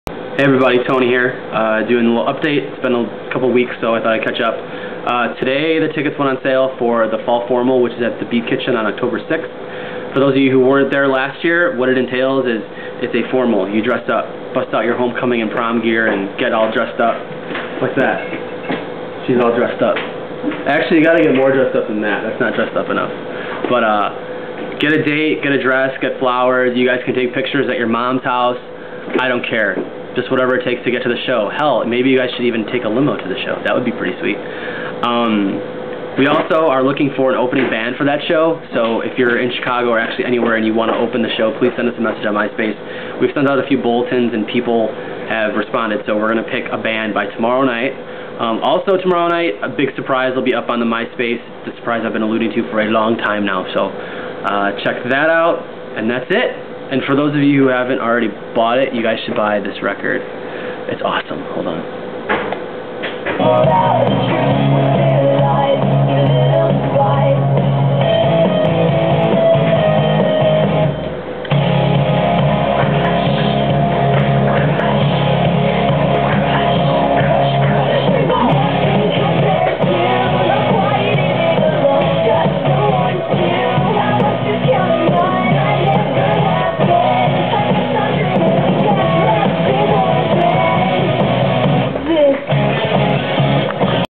STATE AND MADISON UPDATE: 09.08.07
Fall Formal at the Beat Kitchen info, and special announcements.